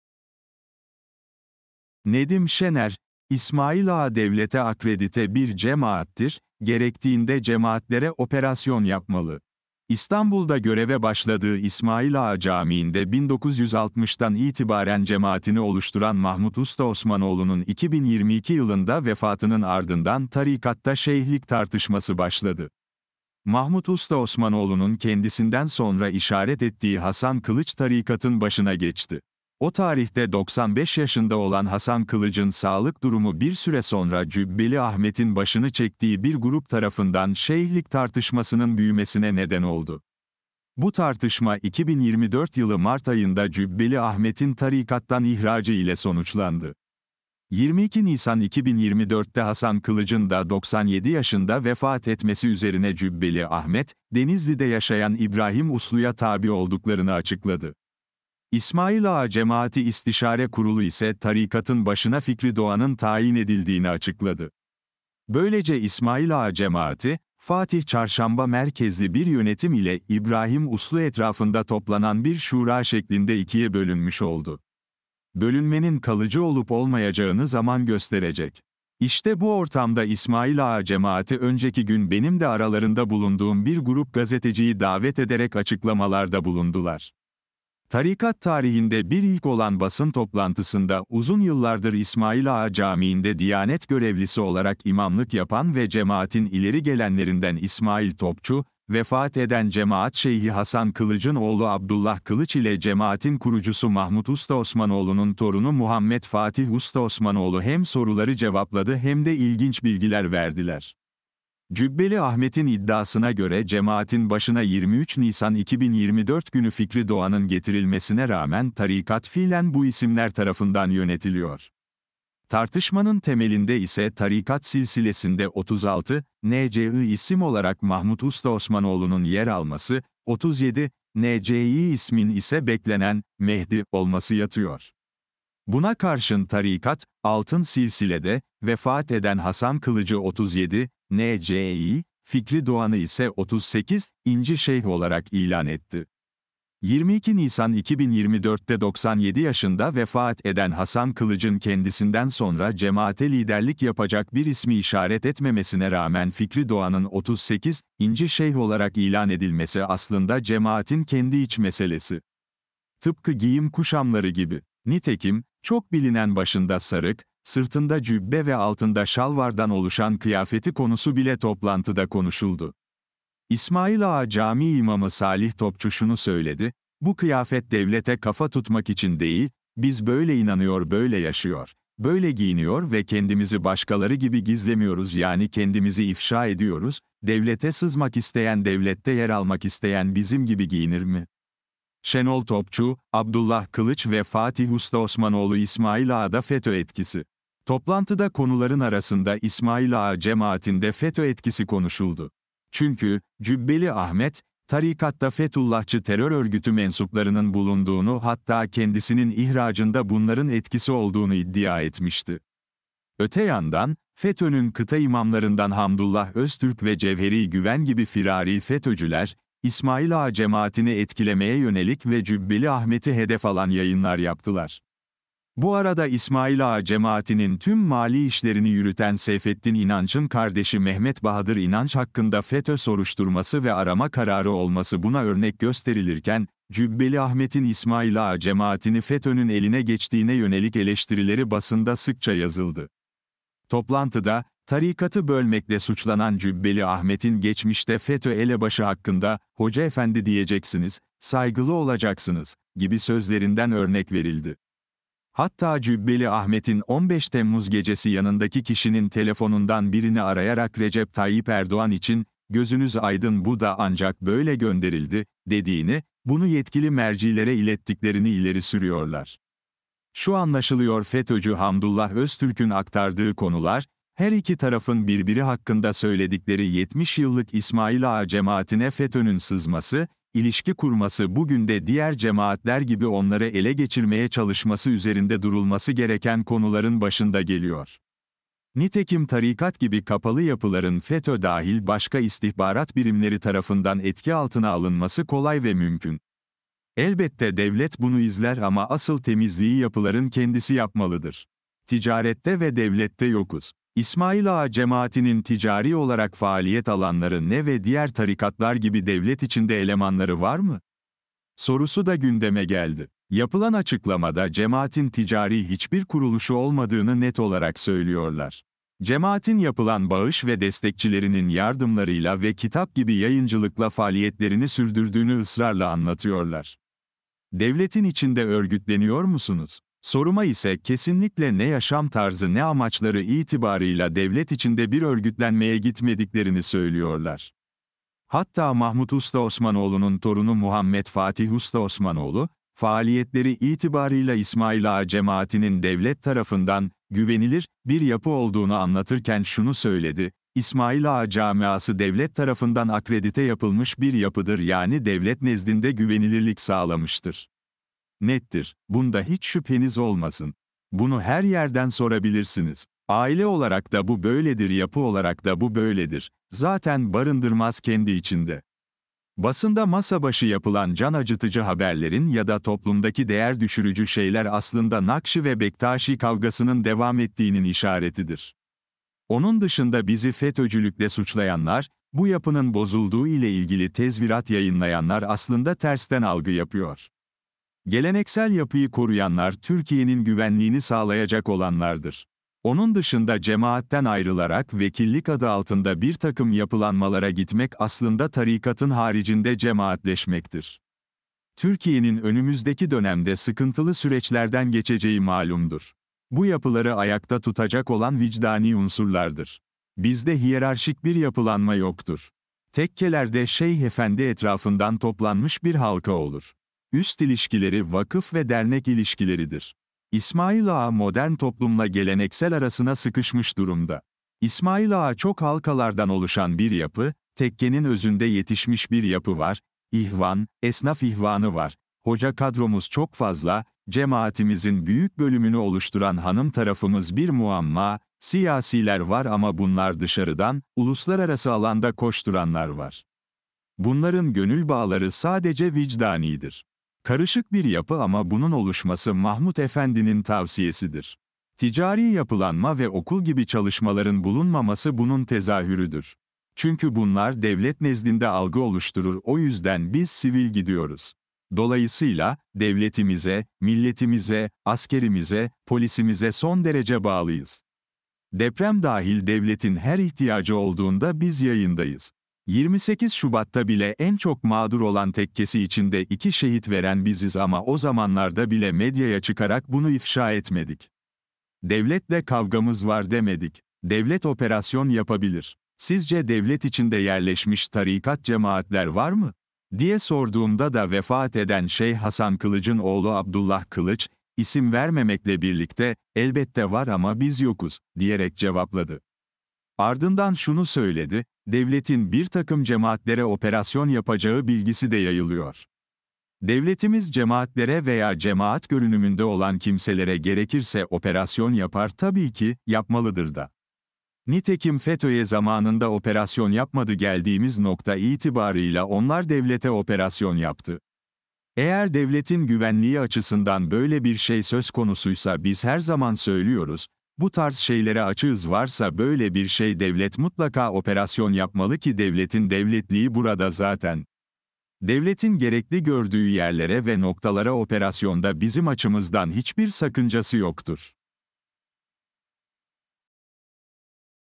NEDİM ŞENER: ‘İSMAİLAĞA DEVLETE AKREDİTE BİR CEMAATTİR’... ‘GEREKTİĞİNDE CE... - SESLİ MAKALE DİNLE
Nedim Şener - hurriyet.com.tr - ‘İsmailağa devlete akredite bir cemaattir’... ‘Gerektiğinde cemaatlere operasyon yapmalı’ - 3 Mayıs 2024 tarihli yazısının seslendirmesi

"İstanbul’da göreve başladığı İsmailağa Camii’nde 1960’tan itibaren cemaatini oluşturan Mahmut Ustaosmanoğlu’nun 2022 yılında vefatının ardından tarikatta şeyhlik tartışması başladı. Mahmut Ustaosmanoğlu’nun kendisinden sonra işaret ettiği Hasan Kılıç tarikatın başına geçti. O tarihte 95 yaşında olan Hasan Kılıç’ın sağlık durumu bir süre sonra Cübbeli Ahmet’in başını çektiği bir grup tarafından şeyhlik tartışmasının büyümesine neden oldu. Bu tartışma 2024 yılı mart ayında Cübbeli Ahmet’in tarikat. "